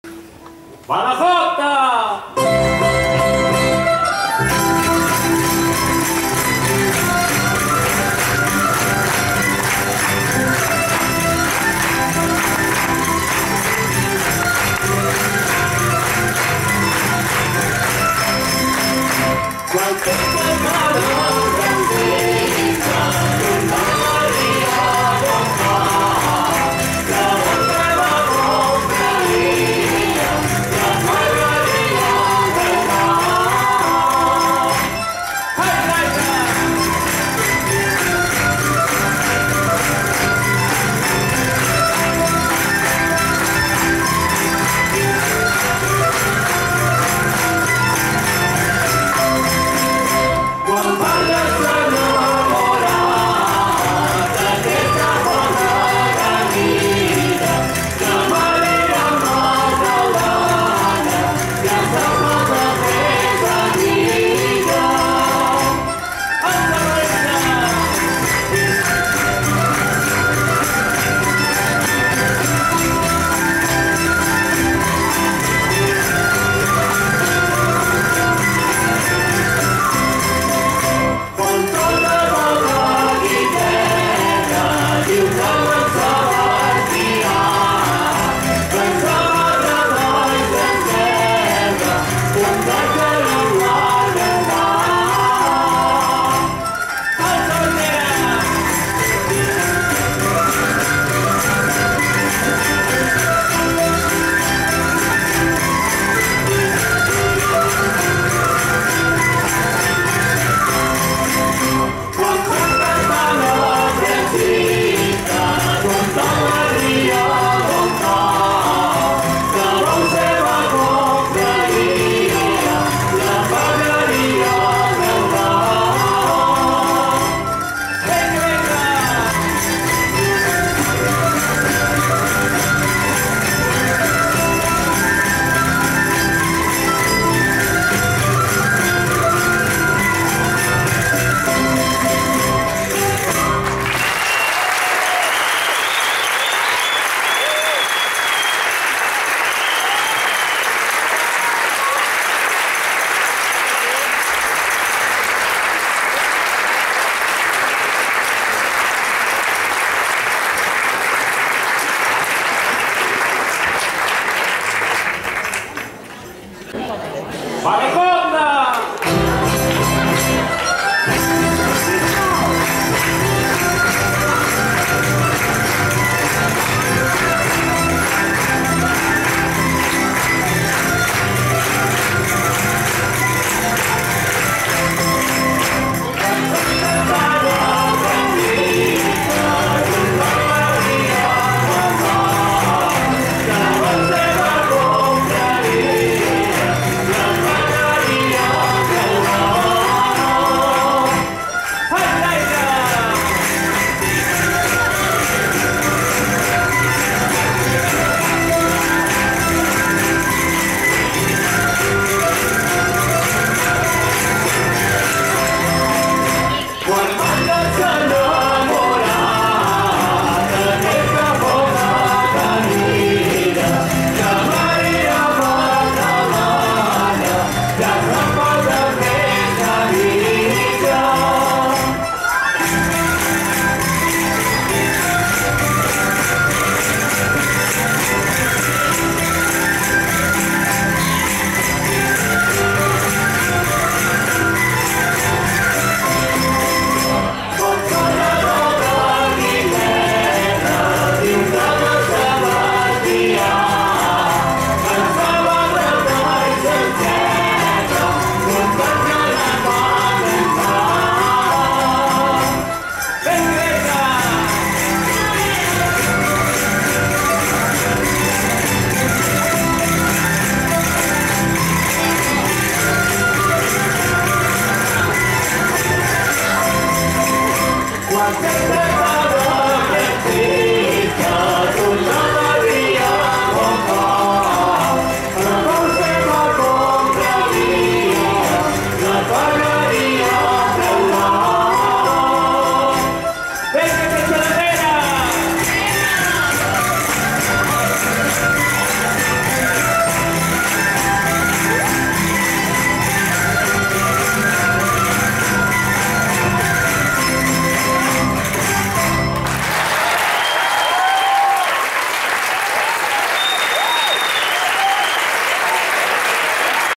Aires gabellins i Castell de Capdepera plegats en els Premis Cap Vermell 2104
Com a punt i final de la gala dels Premis Cap Vermell les dues agrupacions sonen i ballen plegades.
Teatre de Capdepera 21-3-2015